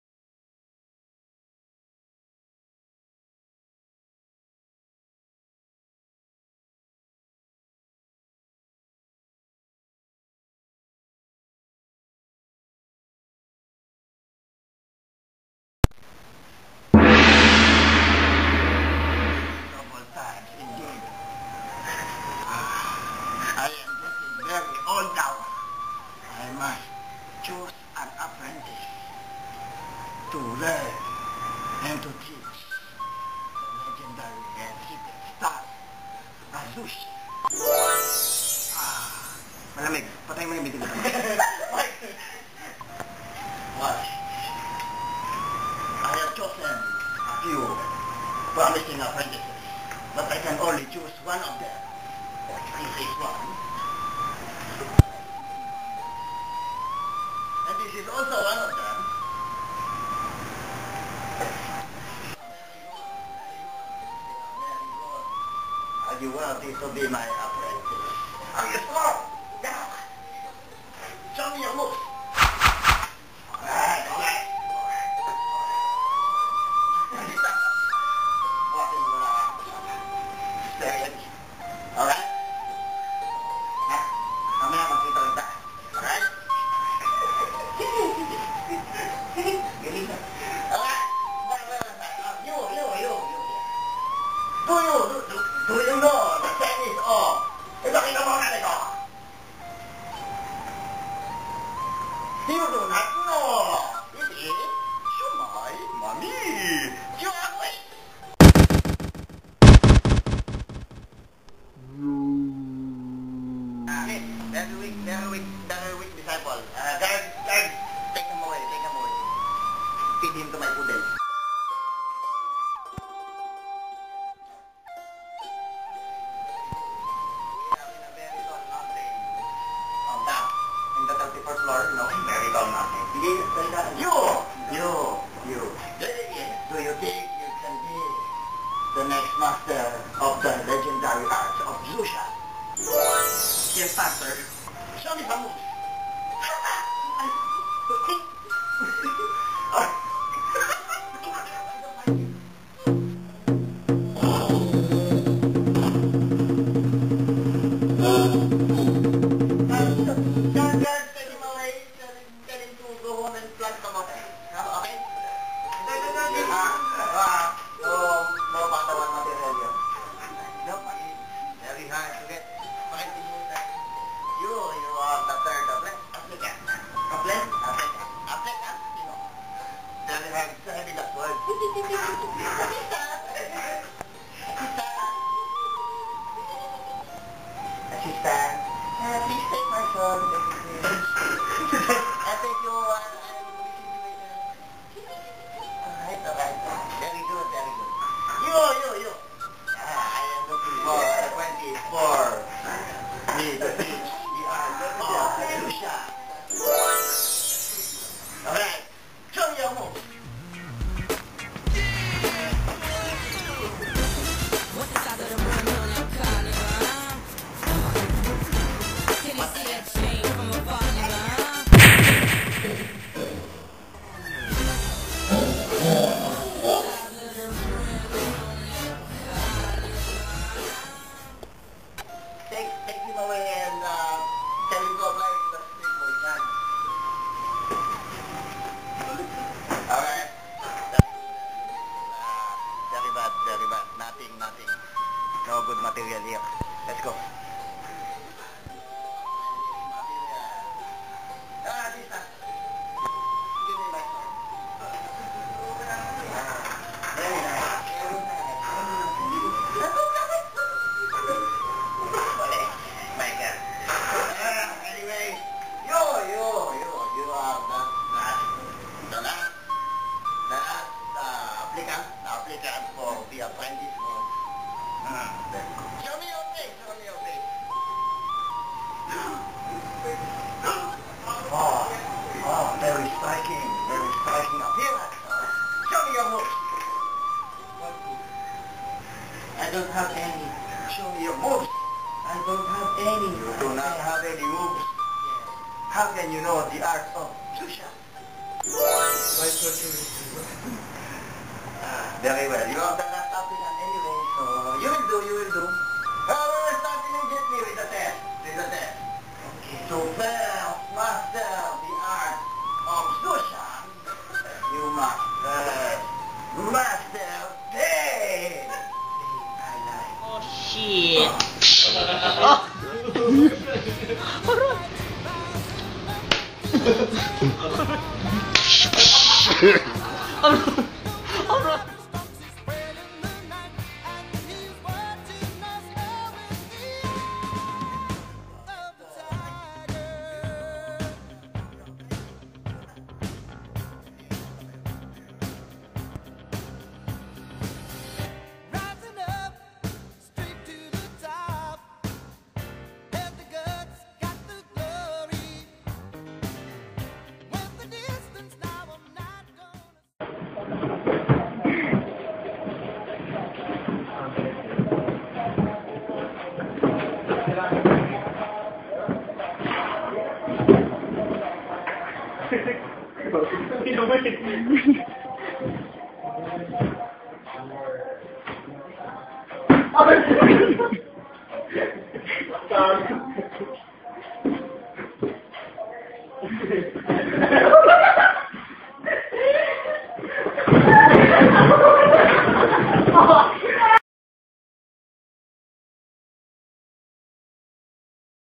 ZX Episode 1
Master Zhu looks for his apprentice. testing 3 aspiring disciples...